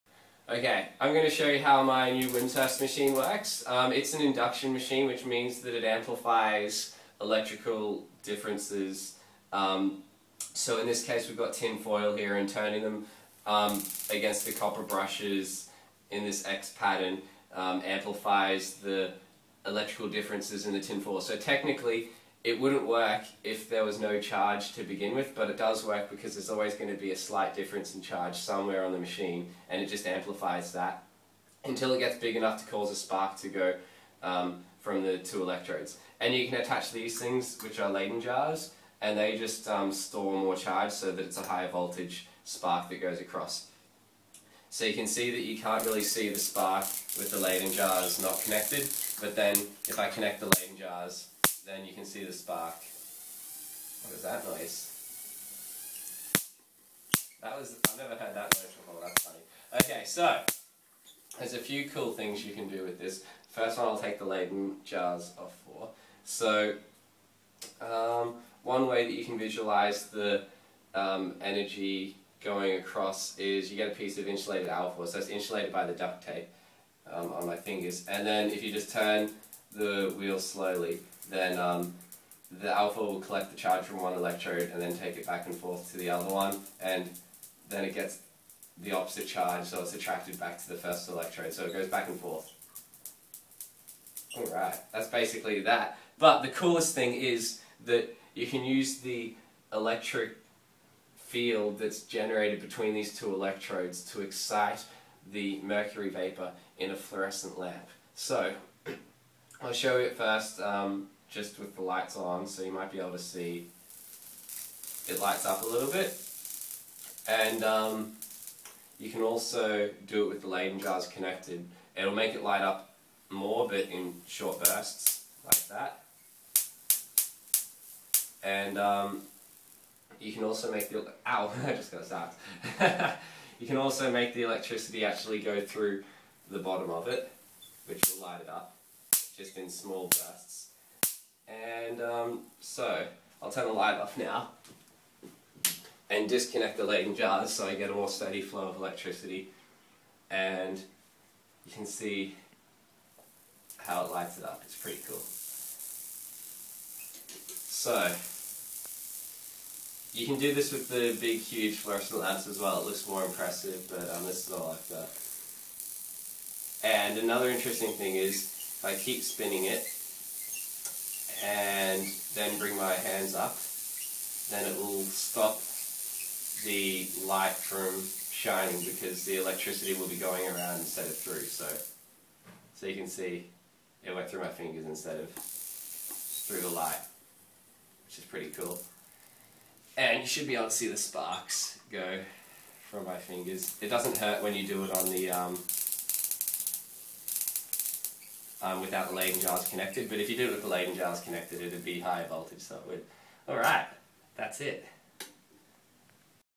Wimshurst Machine Experiments - Lighting Light Bulb Without Contact
Some experiments with a high voltage induction machine.